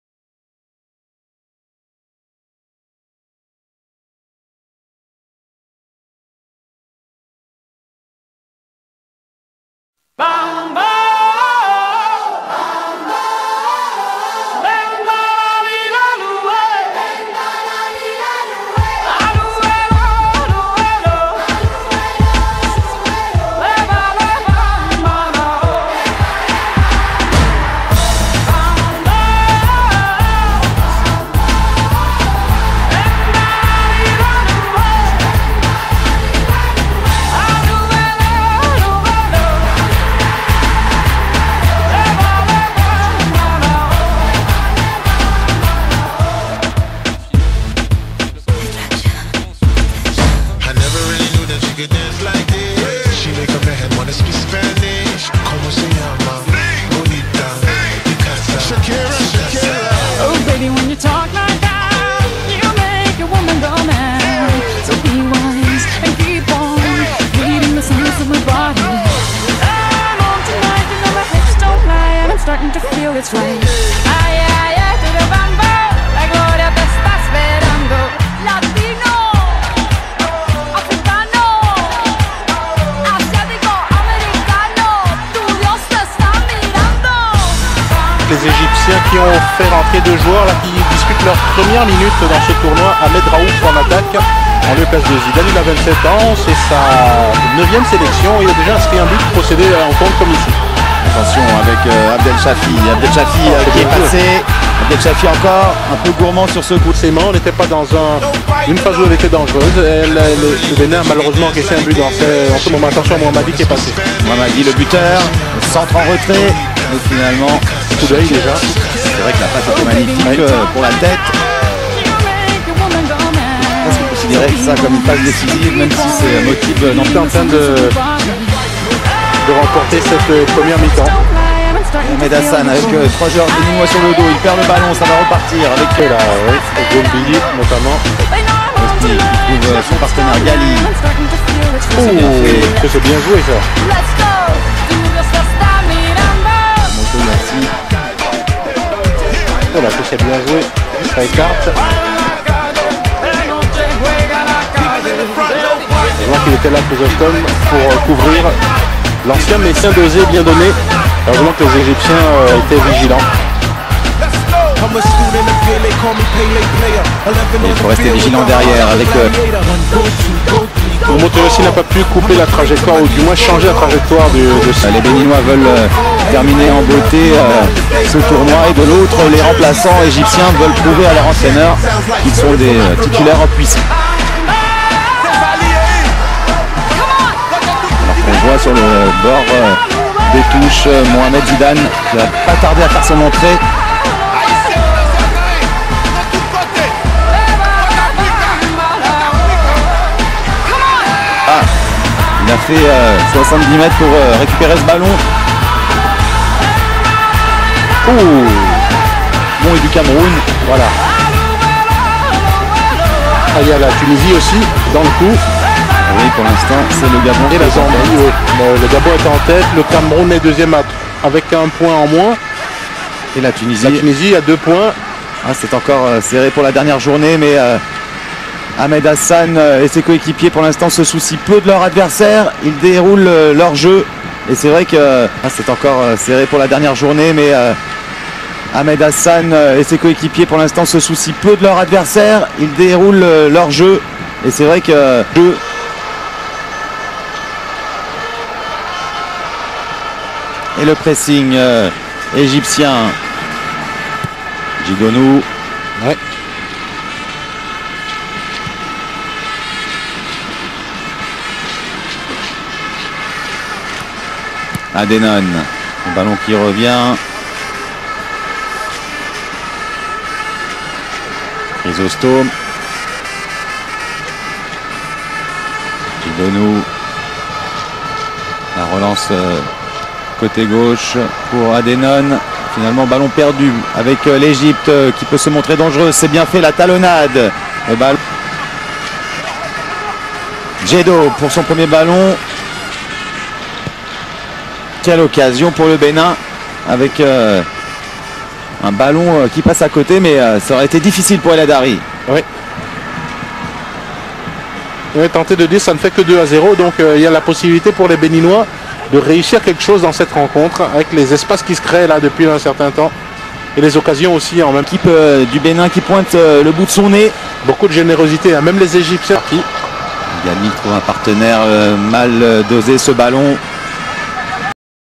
Damien Koffi Chrysostome - Highlights
Damien Chrysostome - Defender - capitan of the National team of Bénin